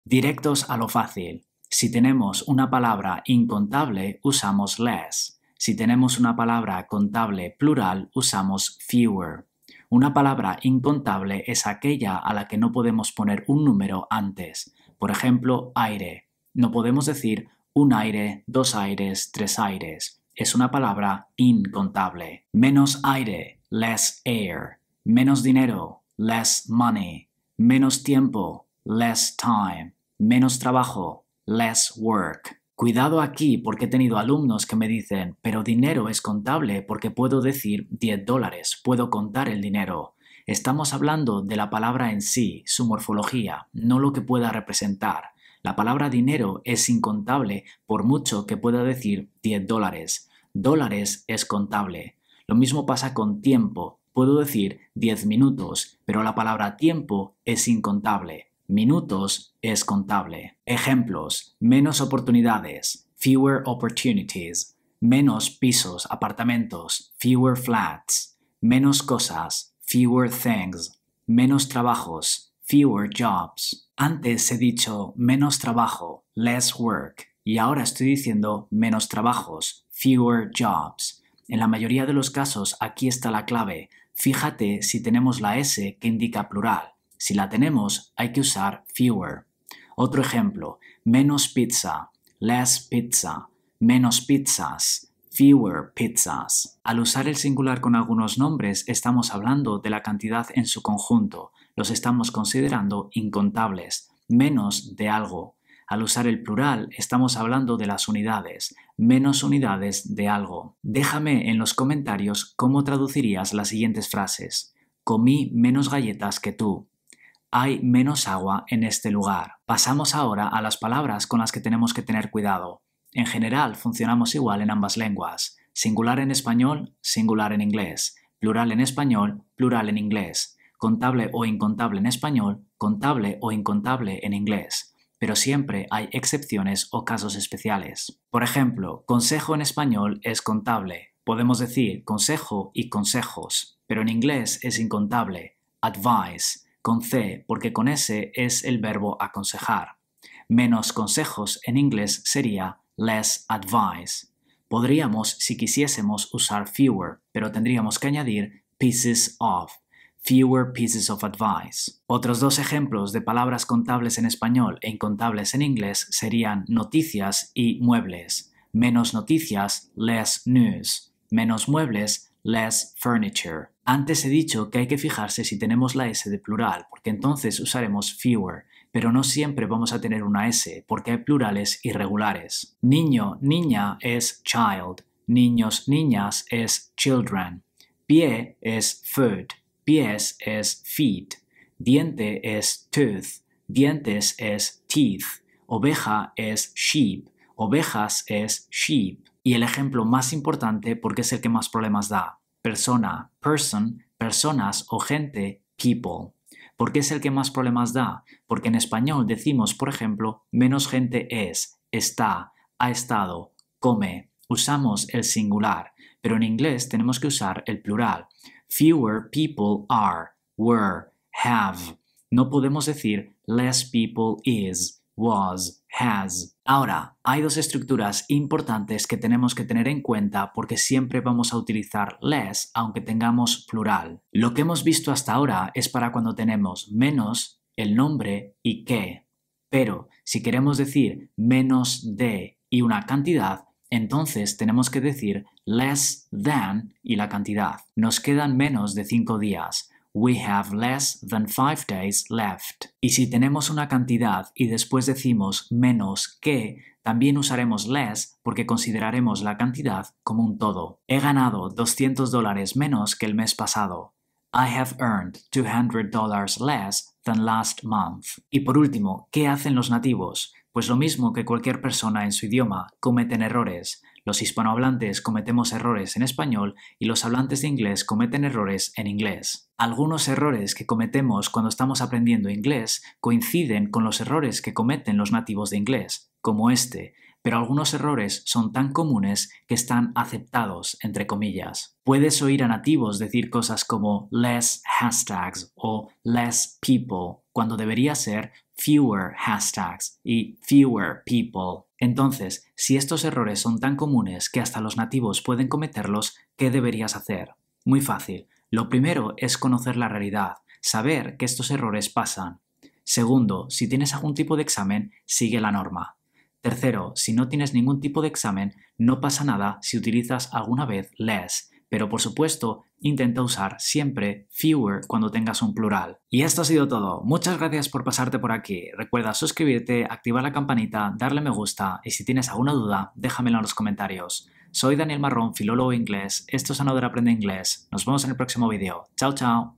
Diferencia entre LESS y FEWER | Explicación en español SENCILLA ✅✅✅
Aprende inglés usando "fewer" y "less". Ambas palabras se traducen como "menos", pero usaremos una u otra dependiendo de la palabra que tengamos después y de la estructura que queramos usar.